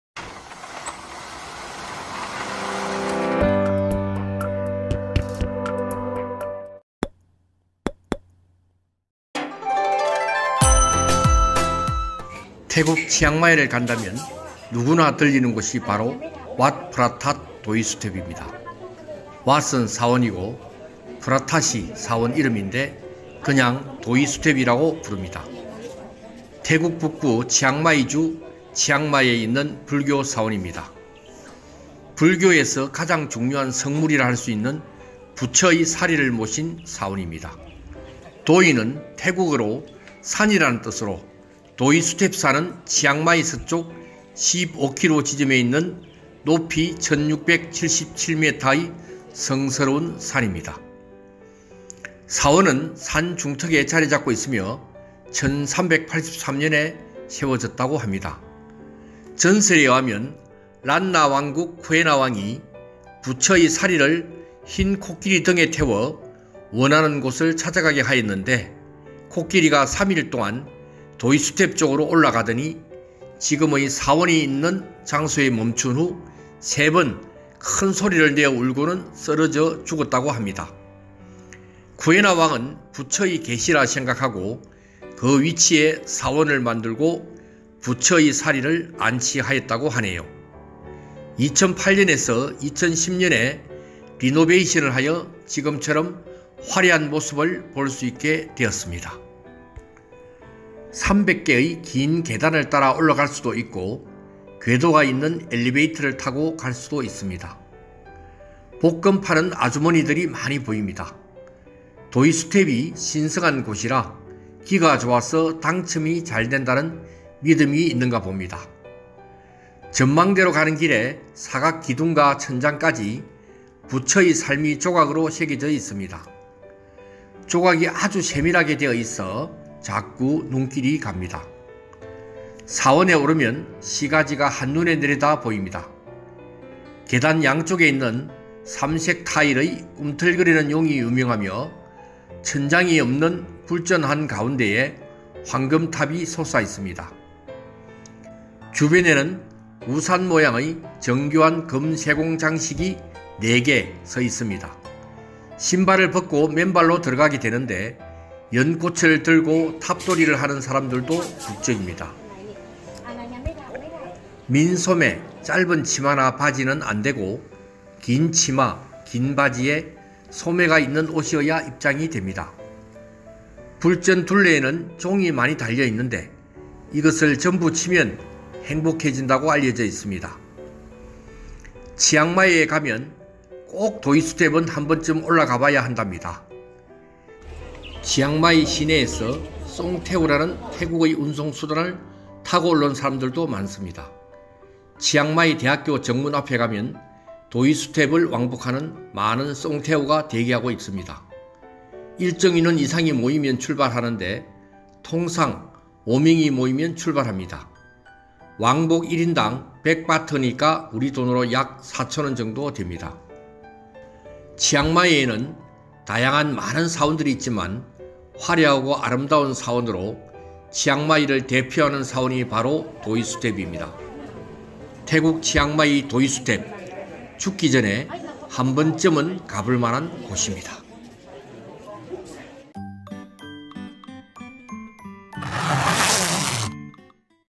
태국 치앙마이의 도이수텝 사원
치앙마이에는 많은 사원들이 있지만 화려하고 아름다운 사원으로 치앙마이를 대표하는 곳이 바로  도이수텝 사원입니다.